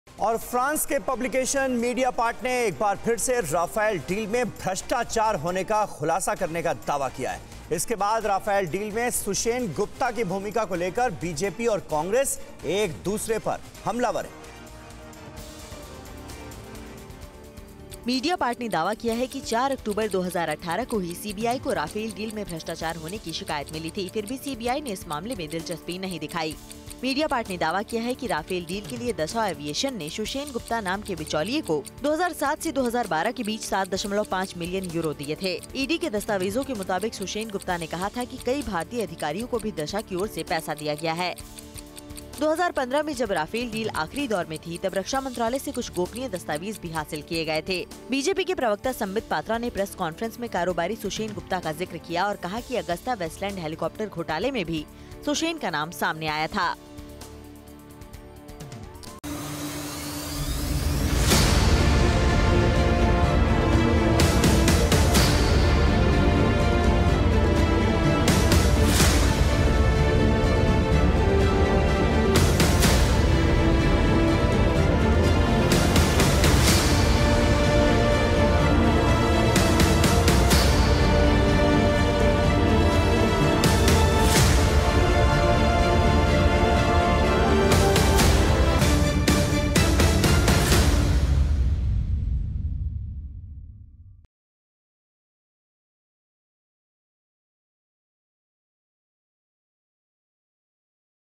Rafale Deal में कमीशन का खेल, सियासी दंगल शुरु
फ्रांस के पब्लिकेशन मीडियापार्ट ने एक बार फिर से राफेल डील में भ्रष्टाचार होने का खुलासा करने का दावा किया है. इसके बाद राफेल डील में सुशेन गुप्ता की भूमिका को लेकर बीजेपी और कांग्रेस एक दूसरे को पर हमले कर रही है.